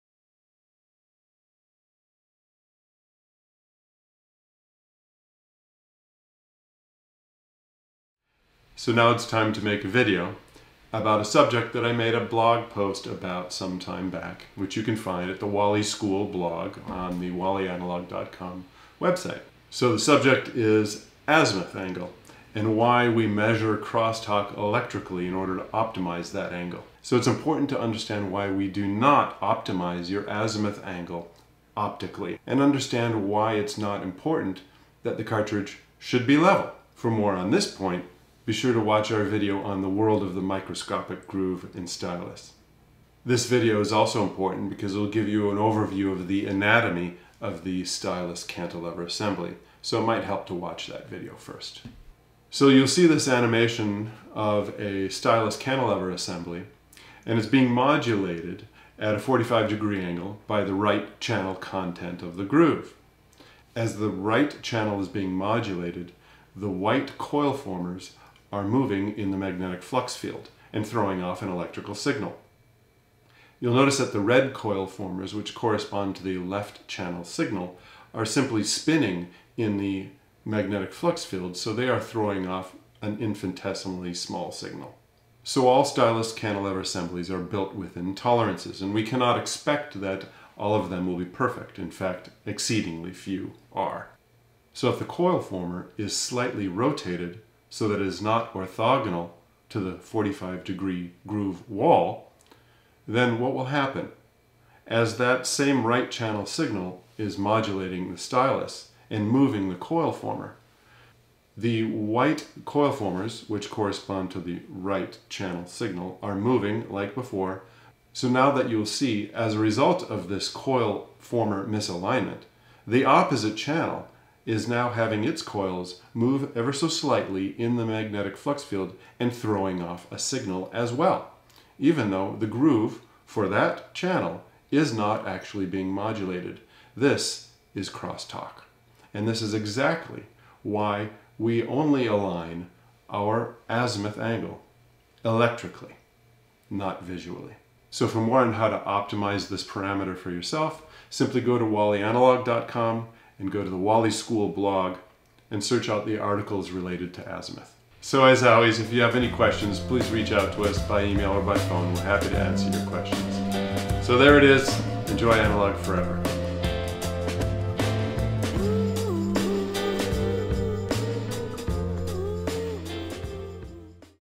Why Measure Cartridge Azimuth Electrically?
There is far too much bad advice out there with regards to aligning our cartridges on the azimuth angle. The purpose we align on the left/right (azimuth) axis is to allow for maximum stereo separation, but this goal has nothing to do with the cartridge body, the cantilever nor even the stylus. Watch this video to see why we must optimize our cartridge on the azimuth axis electrically, not optically.

Learn more about analog optimization on the WAM Engineering website's video